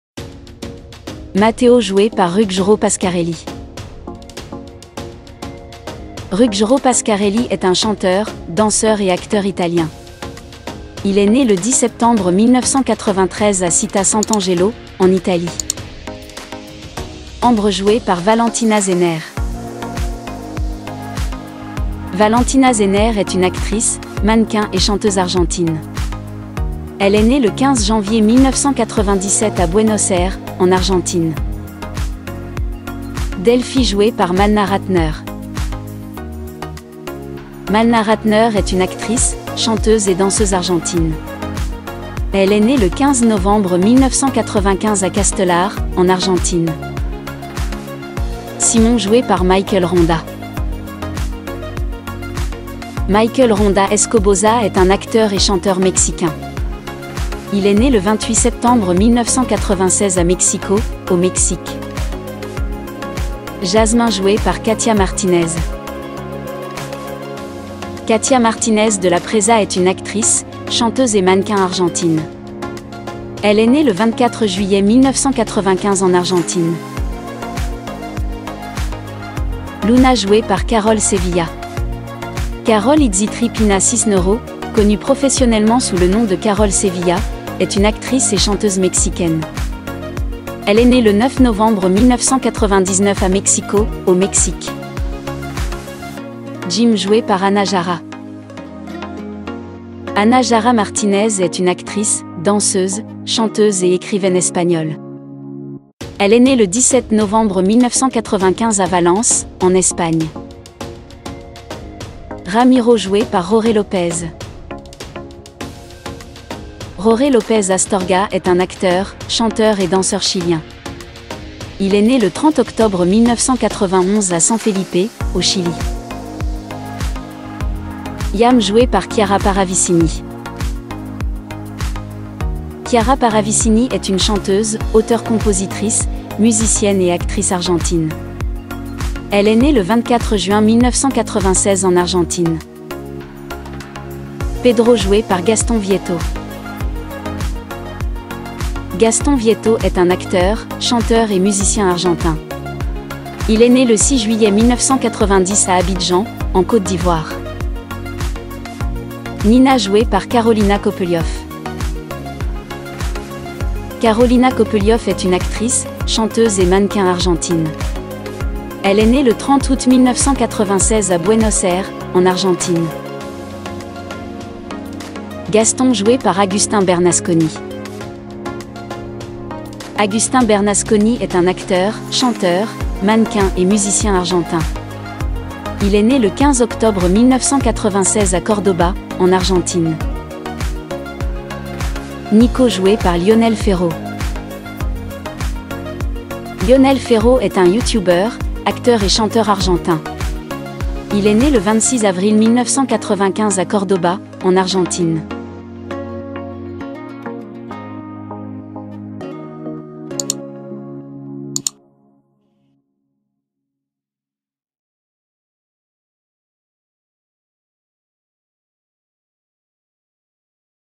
Soy Luna Avant et Après 2023
Où sont-elles maintenant les stars de Soy Luna?

Soy Luna Avant et Après 2023 (La série télévisée Soy Luna)

Dans cette vidéo vous découvrirez la transformation des personnages de la série télé Soy Luna et combien ils ont changé.

Acteurs et actrices de la série Soy Luna Avant et Après 2023 (Soy Luna série télévisée).

Soy Luna est une série télévisée argentine produite par Disney Channel (Amérique latine) et diffusée entre le 14 mars 2016 et le 17 août 2018 en Amérique latine. En France, la série a été diffusée du 18 avril 2016 au 12 octobre 2018.
La saison 1 a été diffusée du 14 mars 2016 au 26 août 2016 en Amérique latine et du 18 avril 2016 au 2 février 2017 en France.
La deuxième saison a été diffusée à partir du 17 avril 2017 au 29 septembre 2017 en Amérique latine et du 24 avril 2017 au 21 décembre 2017 en France.
La troisième saison est diffusée du 16 avril 2018 au 17 août 2018 en Amérique latine et du 23 avril 2018 au 12 octobre 2018 en France.
La première saison est disponible sur Disney+ depuis le lancement de la plateforme en France, le 7 avril 2020. Les deuxième et troisième saisons sont disponibles depuis le 18 septembre 2020 sur la plateforme de streaming.

Les acteurs en français de Soy Luna Avant Après 2023 sont :
Luna Valente - Karol Sevilla (220 épisodes, saison 1 à saison 3)
Matteo Balsano - Ruggero Pasquarelli (220 épisodes, saison 1 à saison 3)
Amber / Ámbar Smith / Ambre Benson - Valentina Zenere (saison 1 à saison 3)
Simón Álvarez - Michael Ronda
Delfina 'Delfi' Alzamendi - Malena Ratner
Jazmín Carbajal - Katja Martínez
Jimena 'Jim' Medina - Ana Jara
Ramiro Ponce - Jorge López
Yamila 'Yam' Sánchez - Chiara Parravicini
Pedro Arias - Gastón Vietto
Nina Simonetti - Carolina Kopelioff
Sharon Benson - Lucila Gandolfo
Reinaldo 'Rey' Gutiérrez - Rodrigo Pedreira
Miguel Valente - David Murí
Mónica Aguilar - Ana Carolina Valsagna
Ricardo Simonetti - Ezequiel Rodríguez
Ana Castro / Dr. Ana Valparaíso - Carolina Ibarra
Mora Barza - Paula Kohan (168 épisodes)
Martín 'Tino' Alcaraz - Diego Alcalá (160 épisodes, saison 1)
Catolino 'Cato' Alcoba - Germán Tripel (160 épisodes, saison 2)
Amanda - Antonella Querzoli (160 épisodes, saison 2)
Juliana / Marisa Mint - Estela Ribeiro (140 épisodes, saison 3)
Alfredo Bilder - Roberto Carnaghi (140 épisodes, saison 3)
Tamara Ríos - Luz Cipriota (81 épisodes)
Emilia - Giovanna Reynaud (71 épisodes)
Benicio - Pasquale Di Nuzzo (72 épisodes)
Gary López - Joaquín Berthold
Eric Andrade - José Andrés Chiluiza Calderón (60 épisodes)
Margarita 'Maggie' García Centurión - Victoria Suárez Battán
Mariano - Tomas de las Heras (40 épisodes)
Fernanda - Sheila Piccolo (19 épisodes)
Rocco - Ignacio Heredia Molini (31 épisodes)
Ada / Eva - Candelaria Molfese (30 épisodes)
Gastón Perida - Agustín Bernasconi
Nicolás 'Nico' Navarro - Lionel Ferro